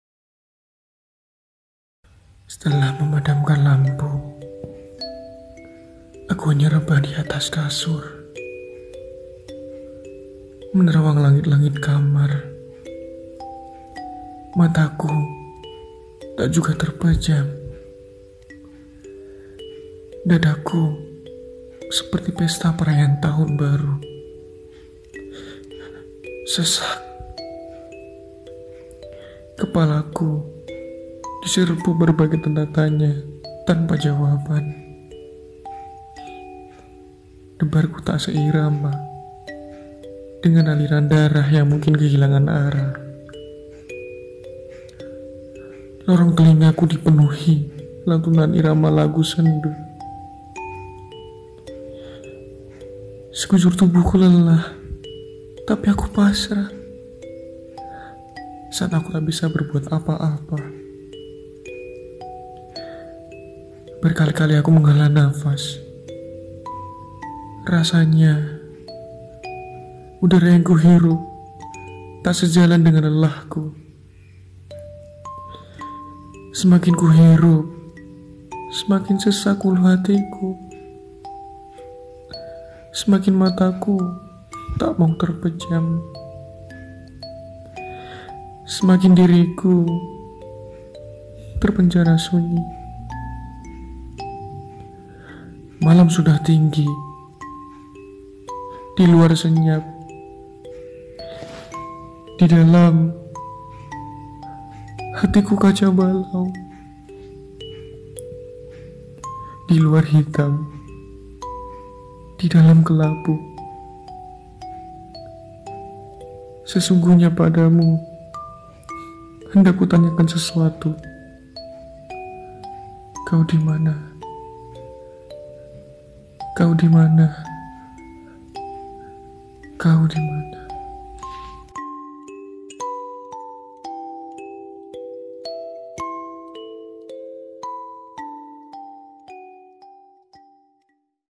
Untuk jiwa yang kacau balau (puisi dari hati)
Terkadang merasa letih dan kosong di dalam jiwa ini masalah masalah menjadi kacau balau menerkam hidup yang penuh dengan drama adakah ketenangan yang di cari dalam cahaya terang di dalam sujud bermohon untuk membuka 1 titik